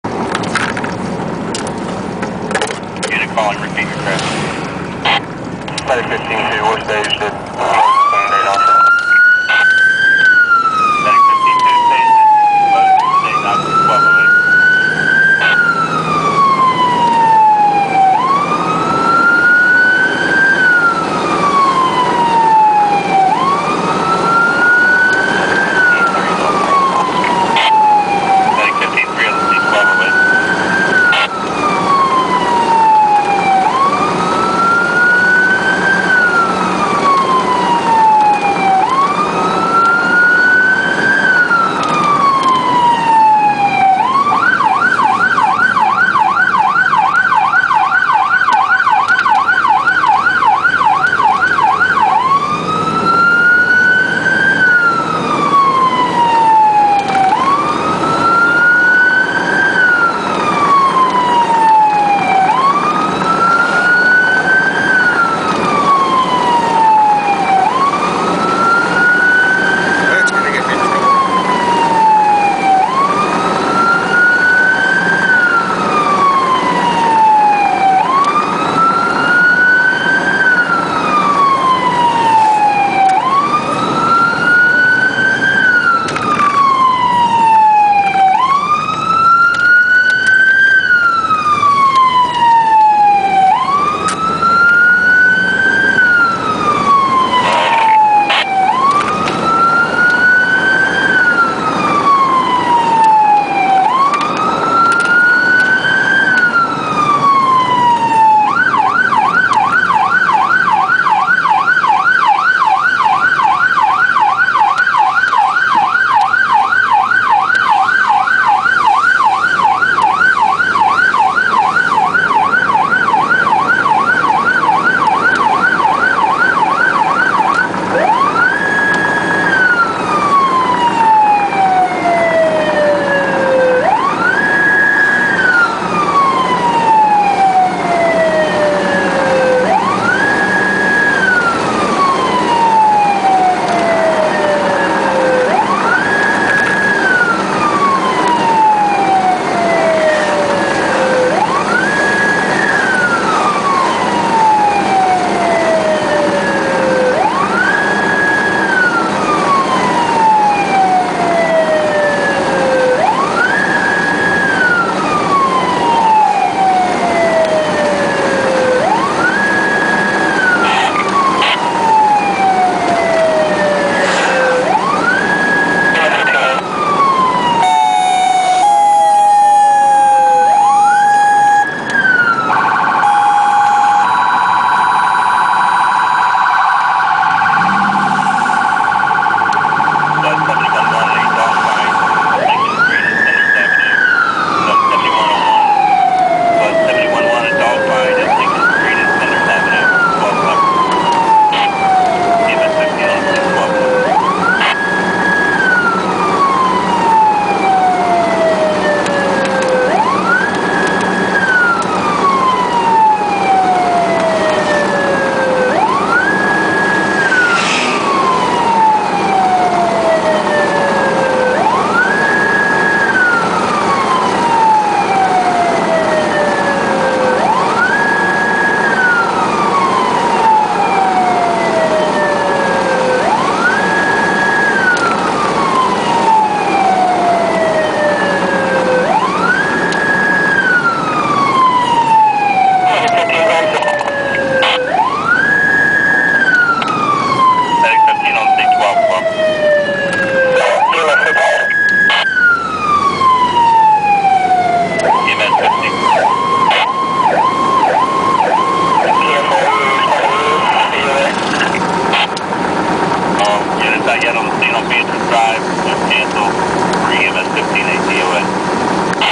POV Response
POV Response to a non-breather canceled enroute